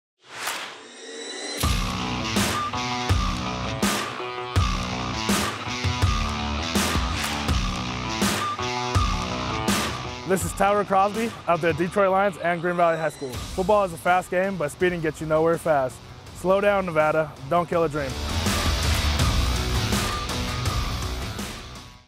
Don’t Kill a Dream: Don’t Speed. Tyrell Crosby PSA
There were 95 speed-related fatalities in Nevada in 2017. That’s 95 dreams lost and left unfulfilled.
Slow down. Don’t Kill a Dream.